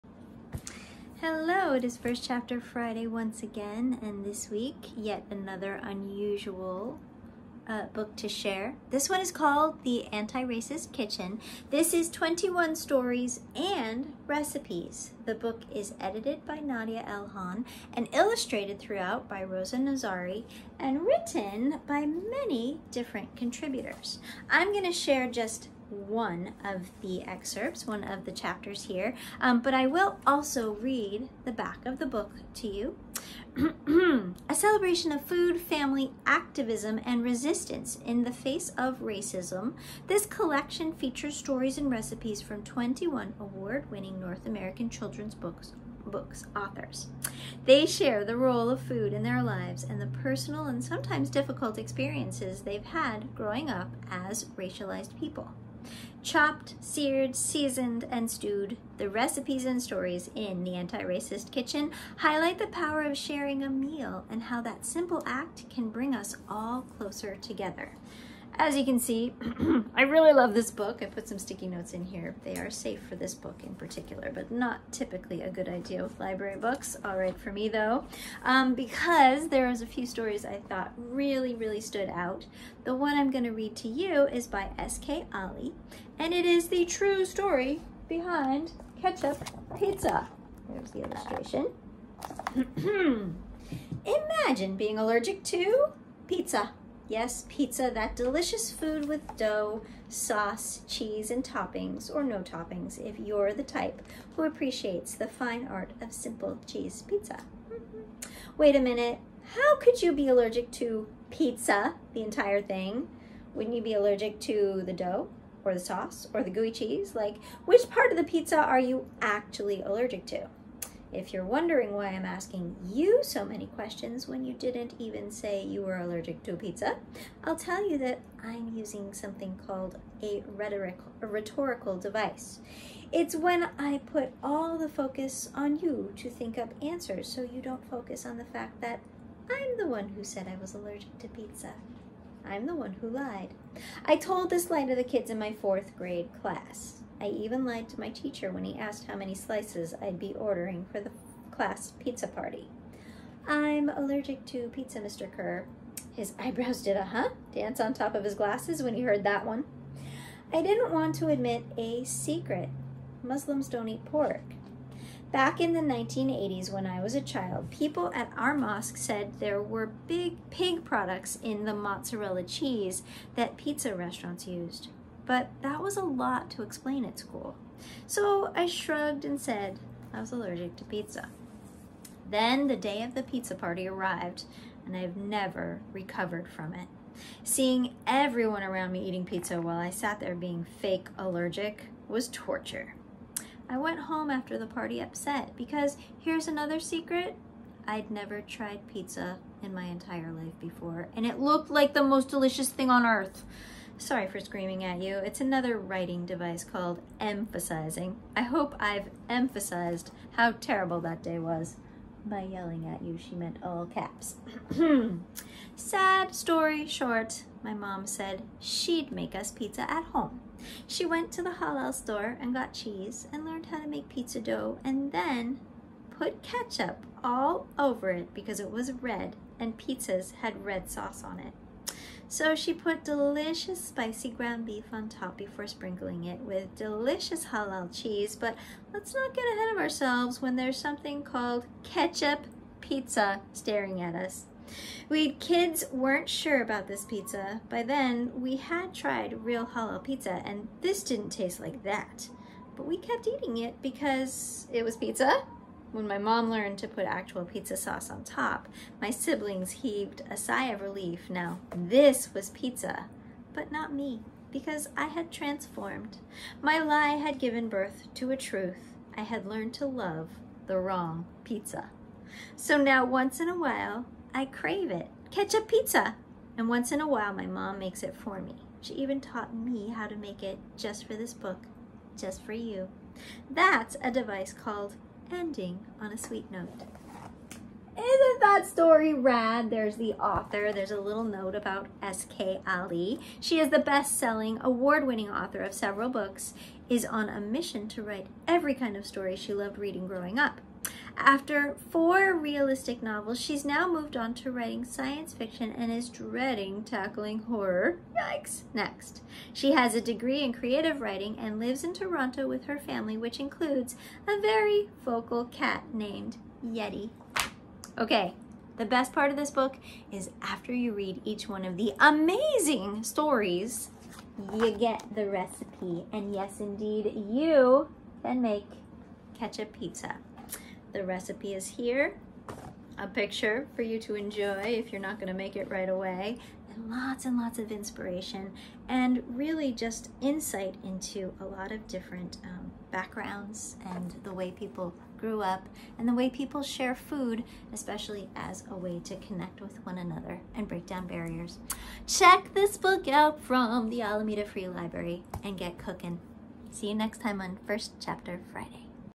Alameda Free Library First Chapter Friday: The Antiracist Kitchen
This book’s got captivating stories and enticing recipes to nourish your mind, body and soul. Check out The Antiracist Kitchen, edited by Nadia L. Hohn and illustrated by Rosa Nozari. It’ll make you think, and it’ll make you hungry!